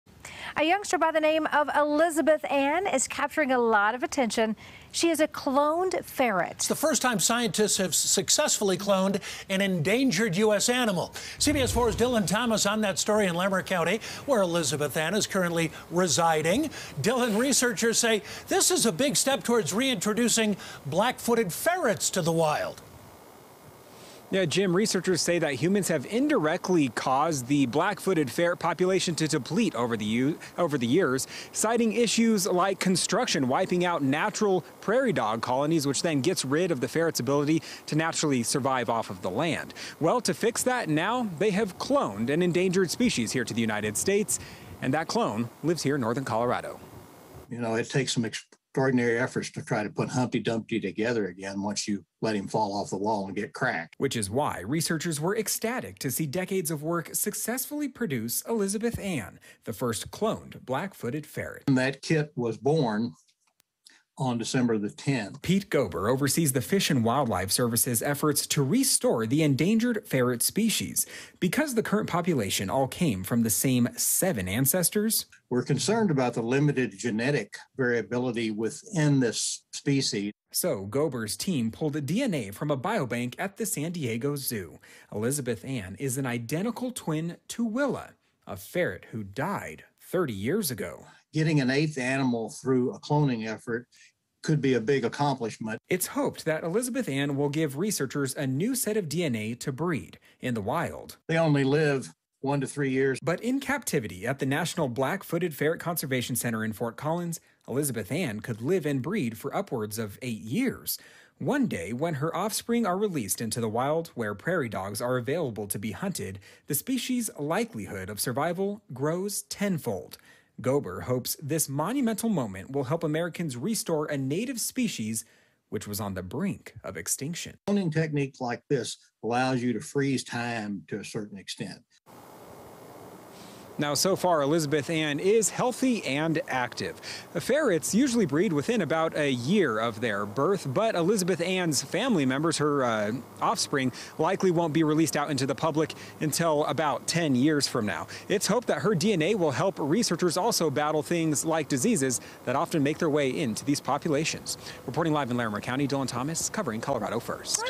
A Black-Footed Ferret Named Elizabeth Ann Is Capturing A Lot Of Attention
Elizabeth Ann is the first successfully cloned endangered animal in the US and her cloning is a big step in saving the black-footed ferret population.